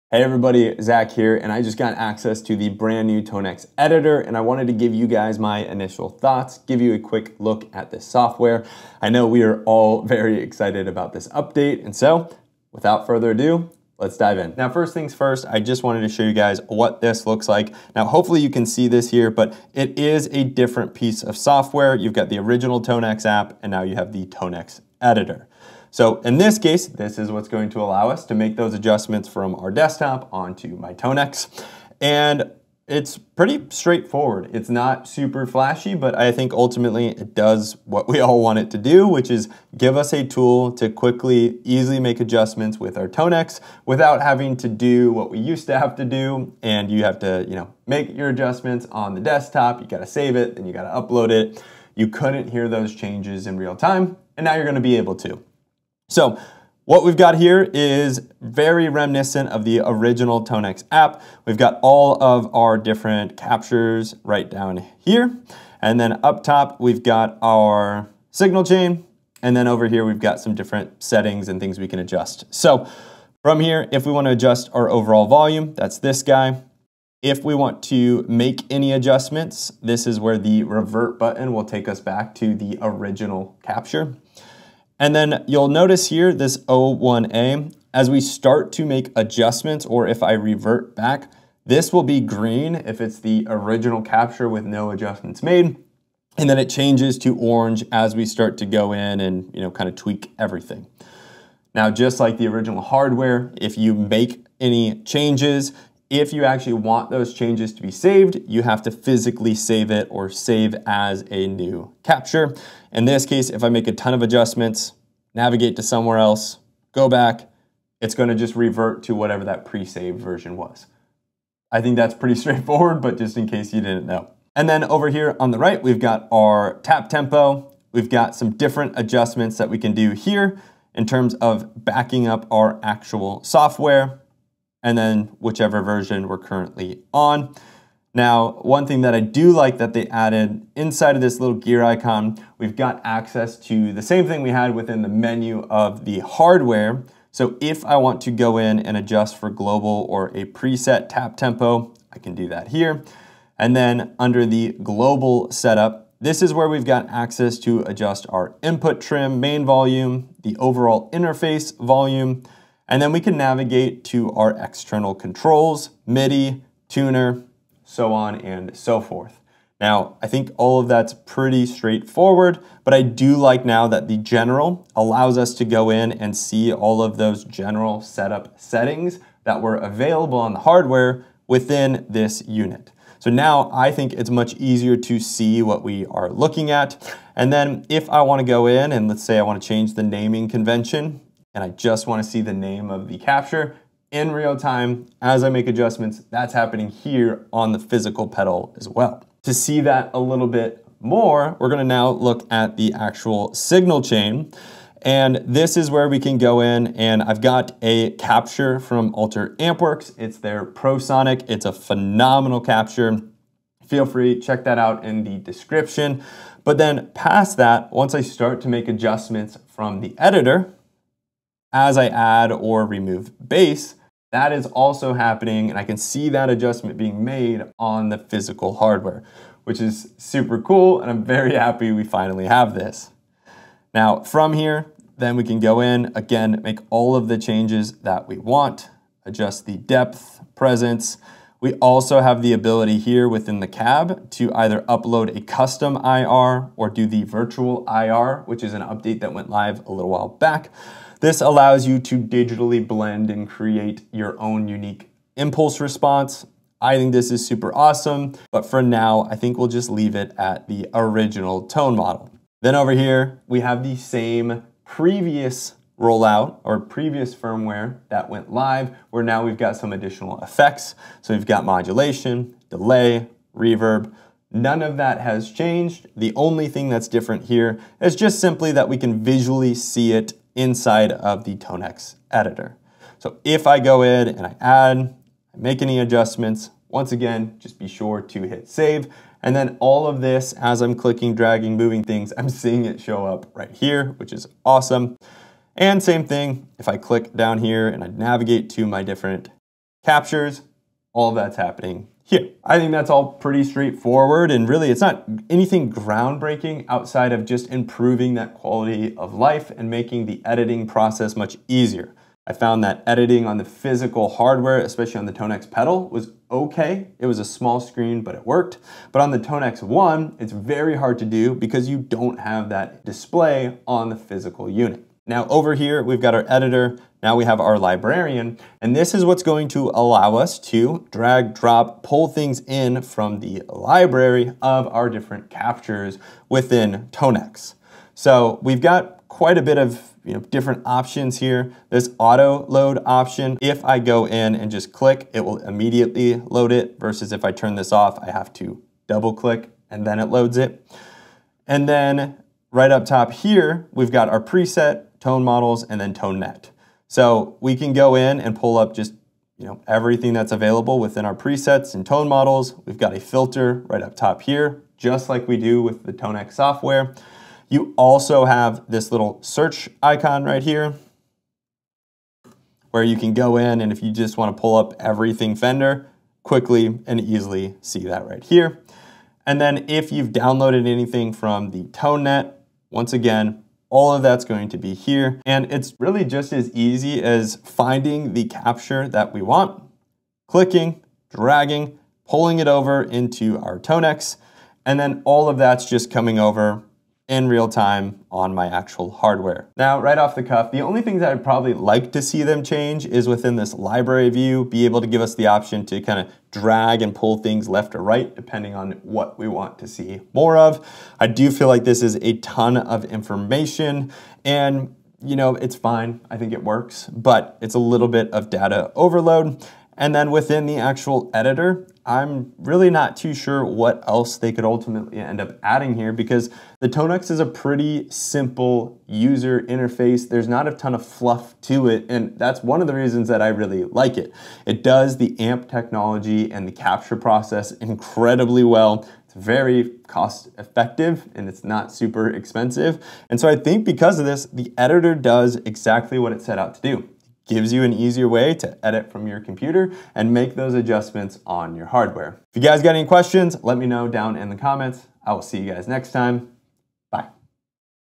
First LOOK at the NEW TONEX Desktop EDITOR!
Hey everyone!

How pumped are you for the new TONEX editor? This has been something that I have been excited about for a while and will certainly make editing presets and captures a whole lot easier.

Please be aware that I am on a BETA version of the software and things may change once this update goes live.

Love the tones you hear on this channel?

(discount will be applied automatically at checkout)